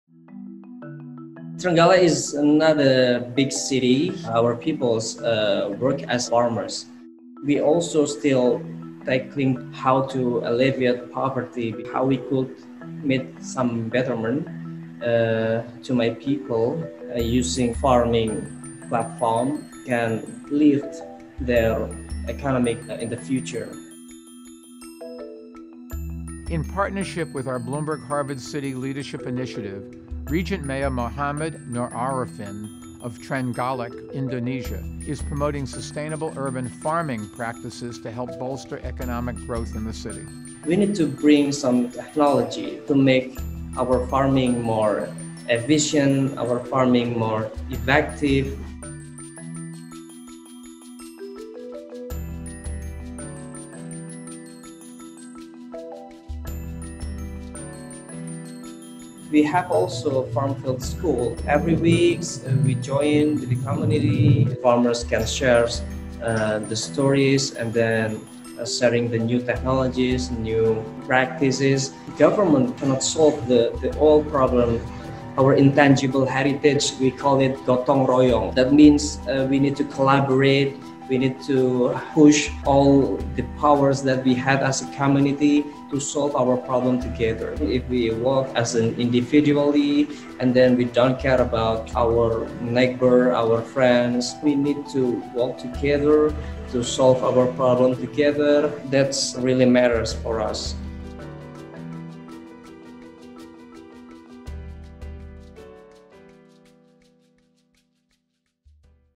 Empowering Indonesian Farmers Through Technology
Working with the Bloomberg Harvard City Leadership Initiative, Regent Mayor Mochamad Nur Arifin of Trenggalek, Indonesia is using innovation, education, and community connections to support sustainable urban farming and expand economic opportunities for his city.